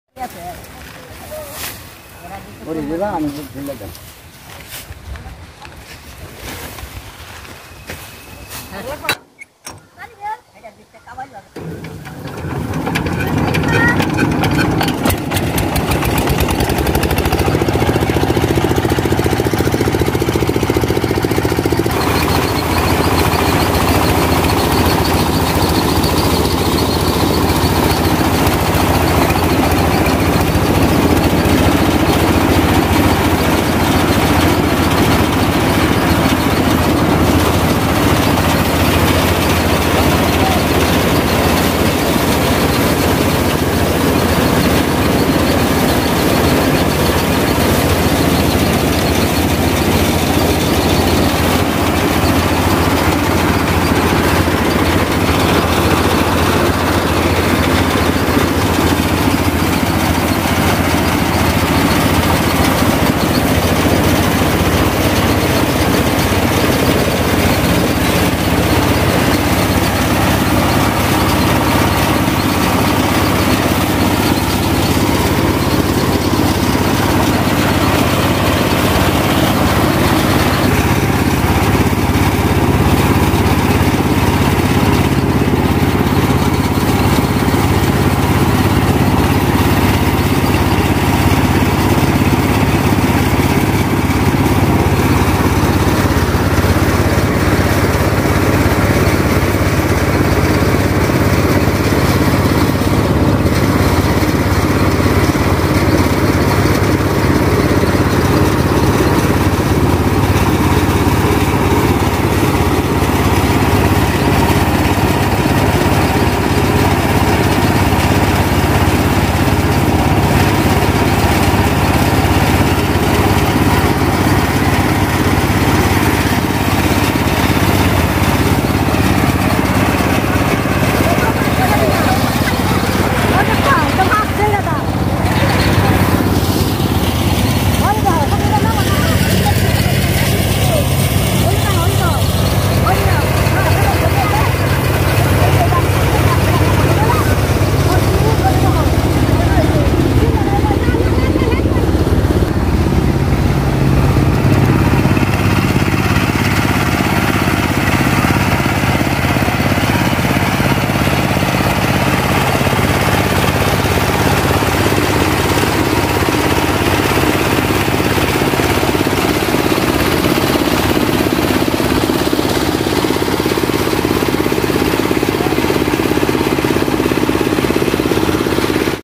rice thresher machine। paddy thresher machine। thresher machine price। ধান মাড়াই মেশিন দেখুন ।
ধান মাড়াই ২০২৪

ধান মাড়াই
আজ আমাদের ধান মাড়াই কাজ চলছে
আমাদের গ্রামে যে ভাবে ধান মাড়াই করা হয়